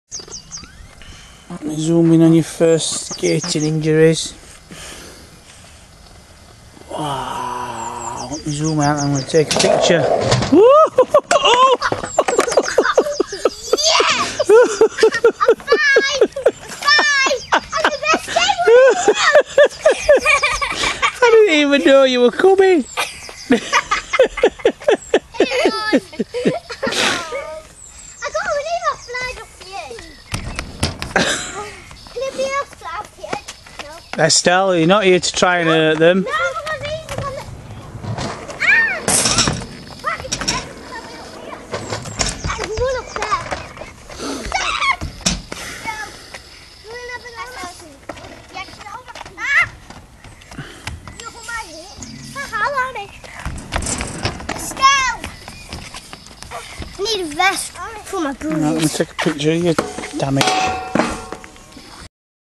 5yr old skater smacked on back of head with board
my five year old son at the skate park with his twin brother learning to skateboard